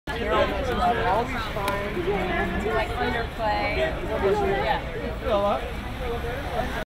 Larry David, Amy Ryan, Greg Mottola at Clear History Los ...
Larry David, Amy Ryan, Greg Mottola at Clear History Los Angeles Premiere on 7/31/13 in Los Angeles, CA .